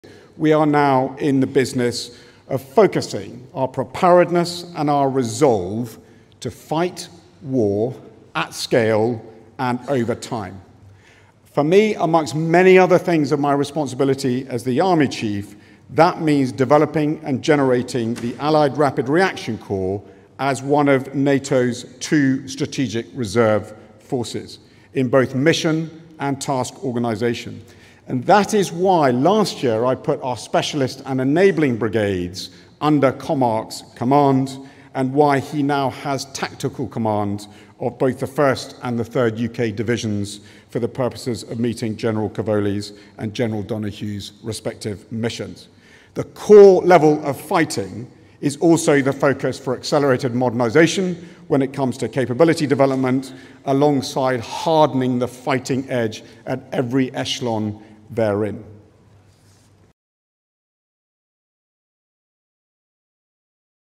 UK Army Chief Outlines Defense Transformation Strategy | Research Engineer
British Army plans to triple fighting power by 2030 through AI-driven modernization and autonomous systems integration.

"We are now in the business of focusing our preparedness and our resolve to fight war at scale and over time. The core level of fighting is also the focus for accelerated modernization when it comes to capability development alongside hardening the fighting edge at every echelon therein." - General Sir Roly Walker.

UK military modernization demonstrates Western commitment to technological superiority against authoritarian threats